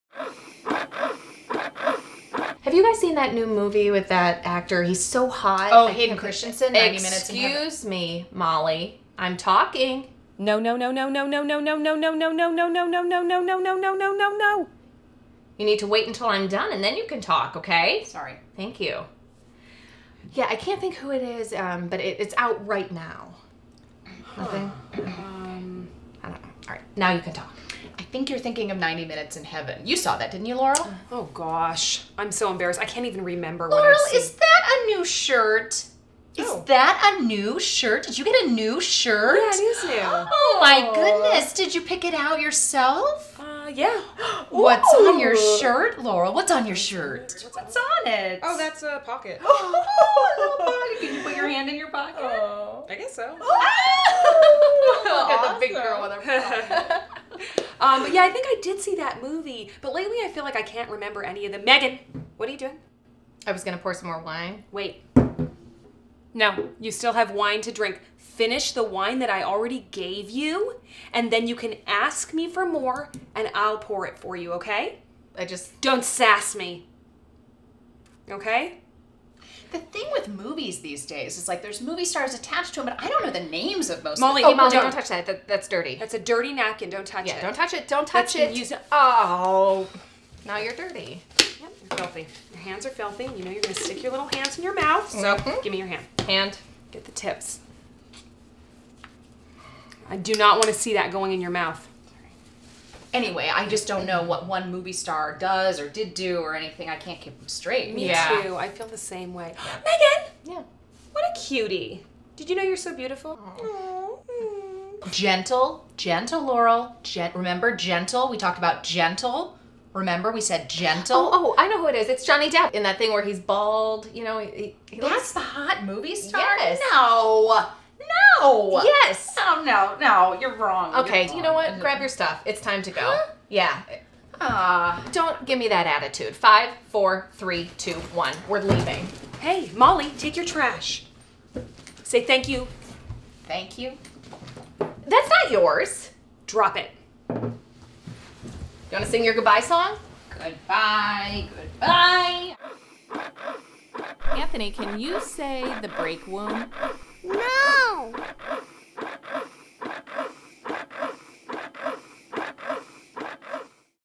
If Moms Talked to Each Other The Way They Talk to Their Kids
The funny BreakWomb mom comedians show what it would look like if moms talked to each other the way they talk to their kids.

Written by & Starring:  Laurel Coppock, Molly Erdman, & Megan Grano
Shot by:    Ryan & Irina Tweedy, Pat Janssen/ @IrinaPhoto @imtweedy
Edited by: Meghan O'Brien